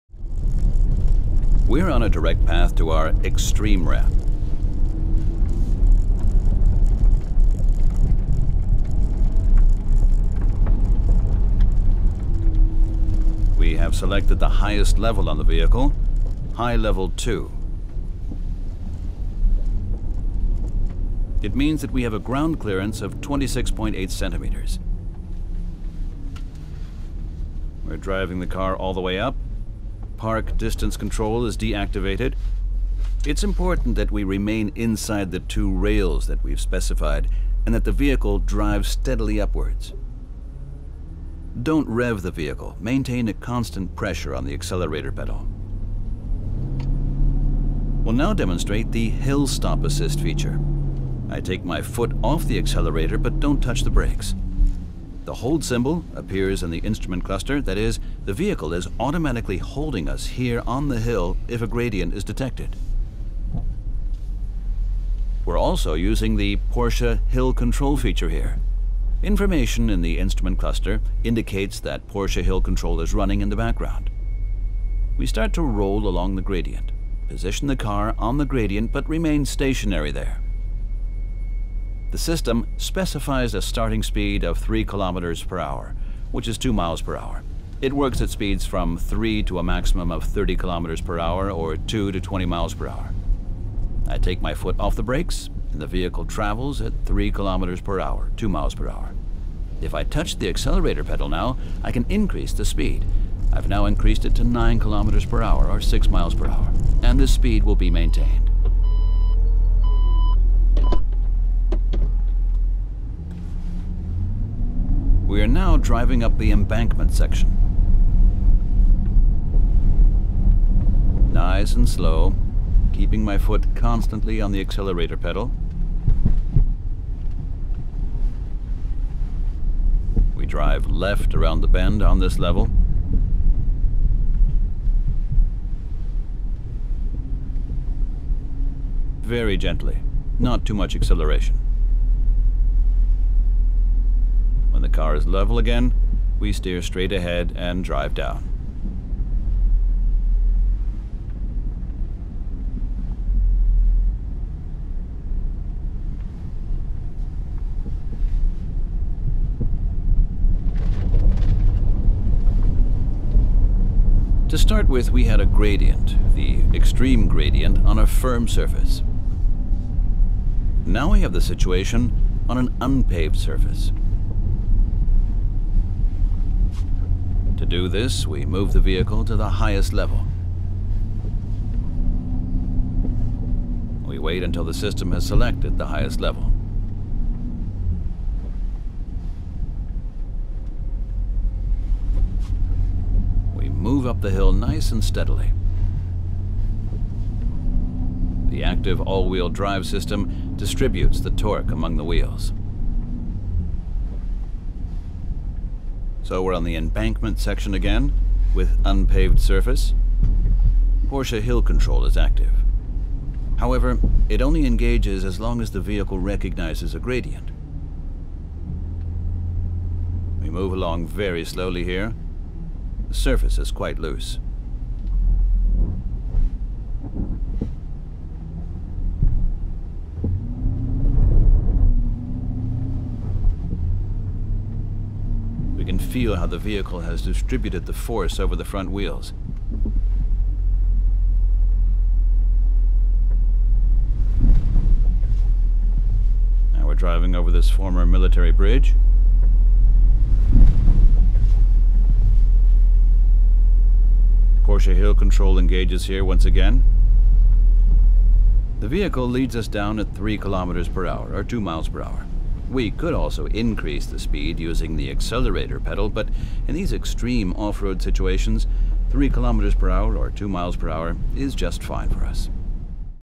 360° Experience - Going Off-Road with the Cayenne Turbo
In this off-road experience video, we’ll take you on the passenger seat of the Cayenne Turbo.

*Combined fuel consumption in accordance with EU 6: Cayenne Turbo 11.5 – 11.2 l/100km; CO2 emissions: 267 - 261 g/km